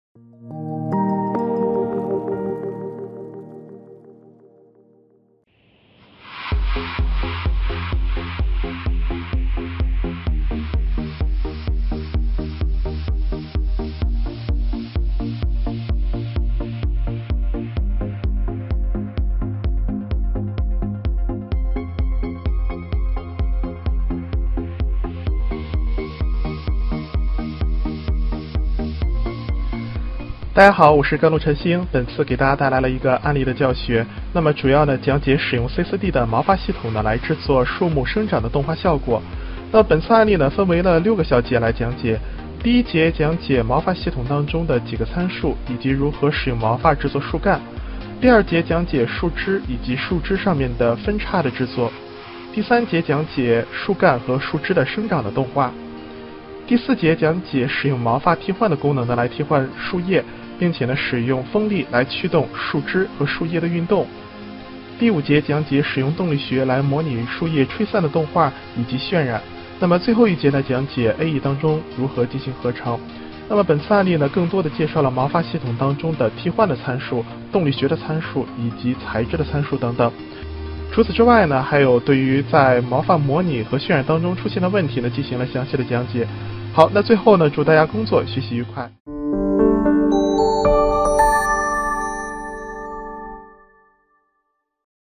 C4D毛发系统制作树木生长实例教程01
本教程主要讲解使用C4D的毛发系统、毛发材质以及动力学来制作大树生长的动画。
对于C4D的毛发系统来说，更多的人都只是知道能够做毛和发，但确确实实毛发系统还可以制作其他效果，比如本节的大树生长、风铃、编织物、床垫中间的弹簧等等。之所以大家了解的比较少是由于大家只知道毛发系统不能生成物体。本次案例会给大家讲解如何使用毛发来生成模型，或者替换模型。除此之外还会讲到C4D中刚体动力学的部分，以及AE当中的合成内容。